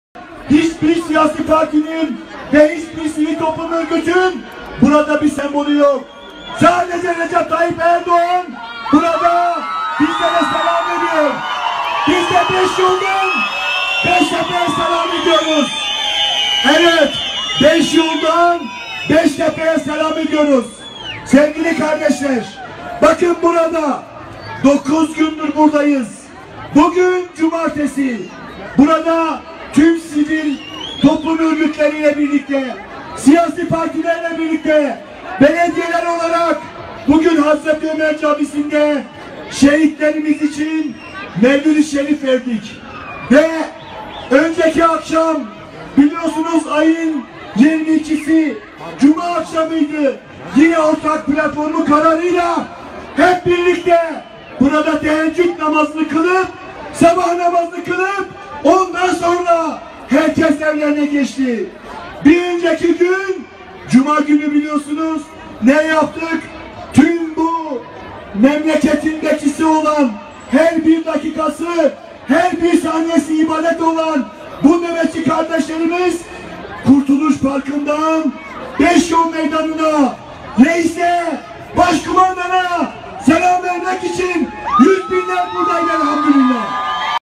Tuşba Belediyesi 195 | Van - Beşyol Meydanında Demokrasi Nöbeti - 9.gün-1 | 23.07.2016 (kısa)
Tuşba Belediyesi Başkanımız Fevzi Özgökçe'nin Beşyol Medanında Halka Hitabı